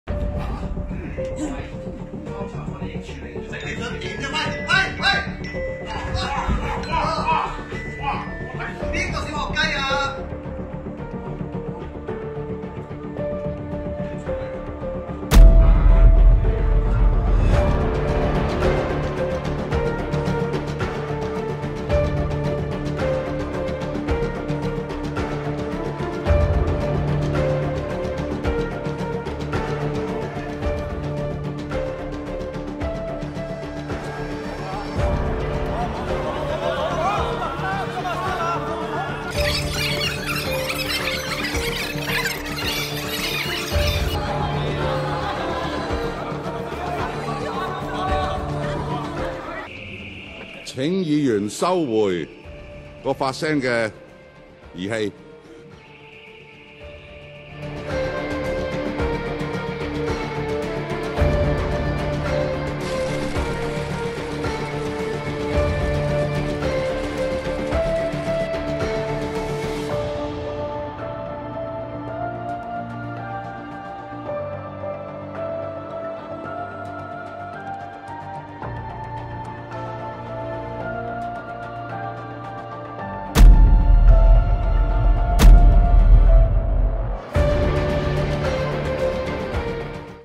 【短片】【議會破壞王】重溫許智峯破壞議會秩序荒謬行徑 強搶手機、襲擊保安、玩防狼器
//由區議會到立法會，佢嘅破壞行徑從來都未停過！//